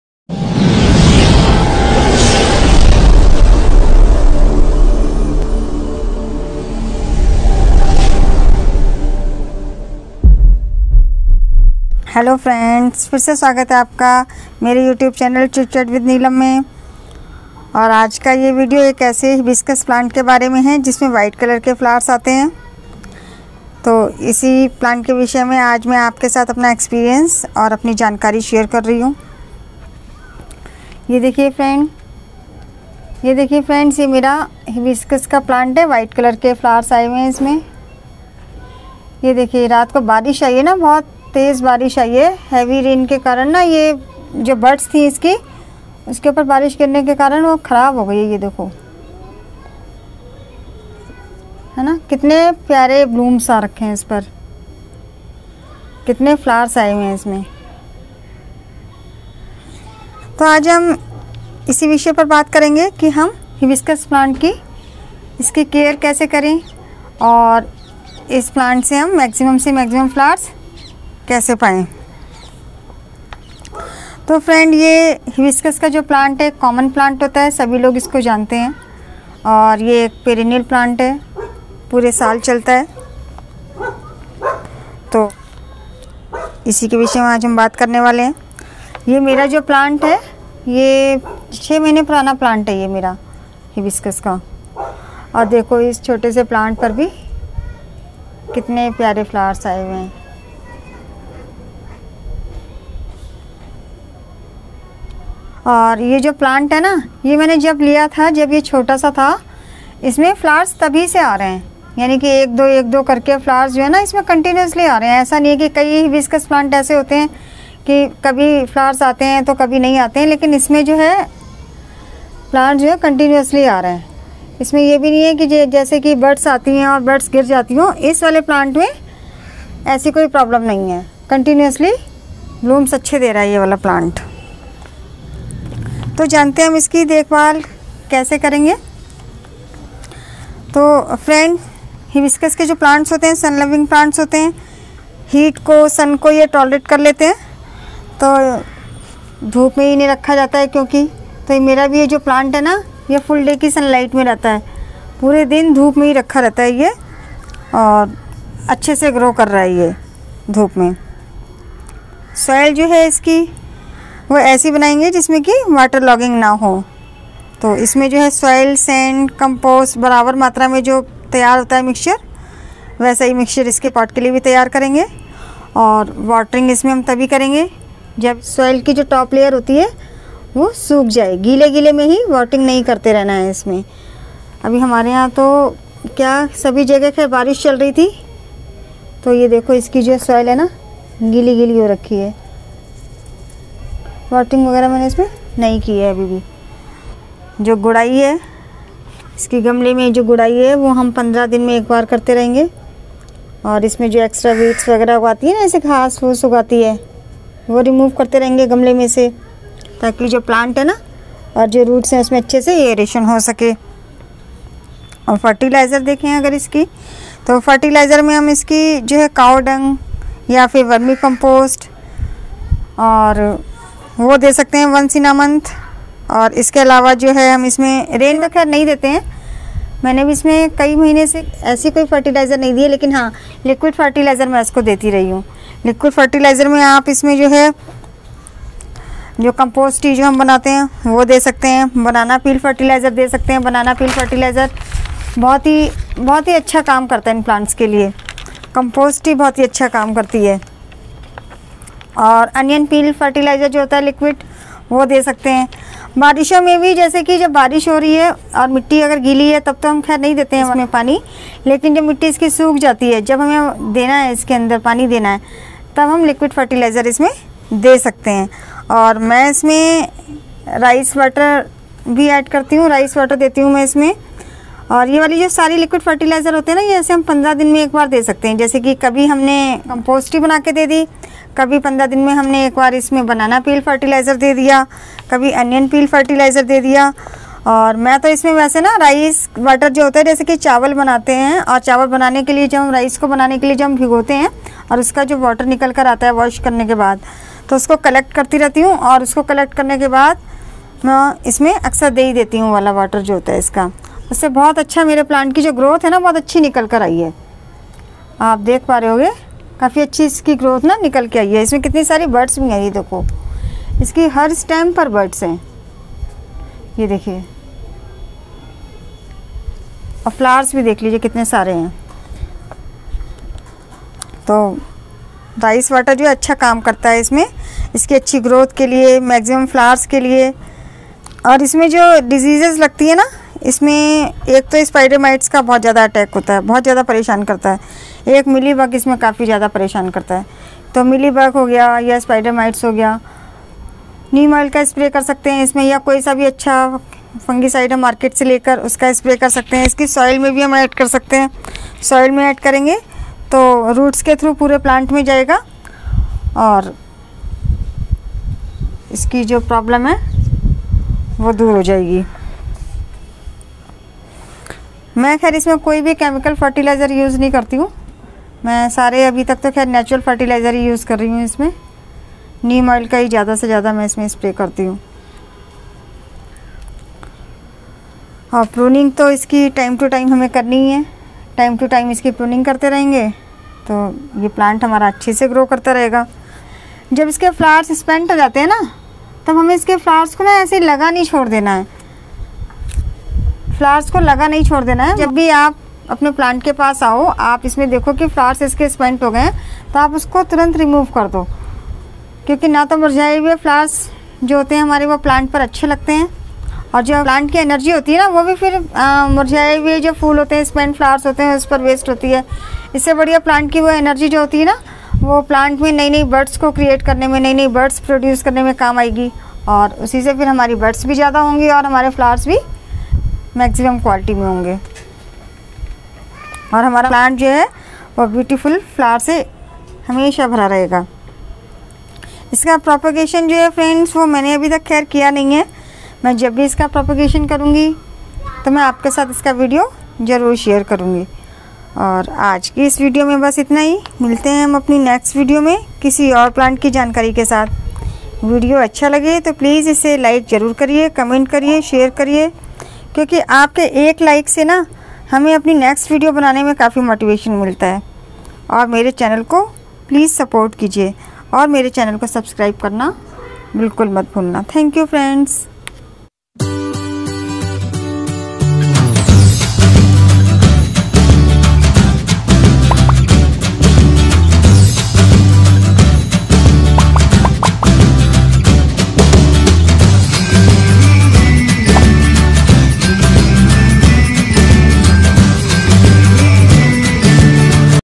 TECHNIQUE OF GETTING MAXIMUM FLOWERS ON HIBISCUS PLANT || CHIT CHAT WITH NEELAM
Technique of getting maximum flowers on Hibiscus Plant ☘️☘️ || Care tips of Hibiscus Plant || Chit Chat With Neelam ||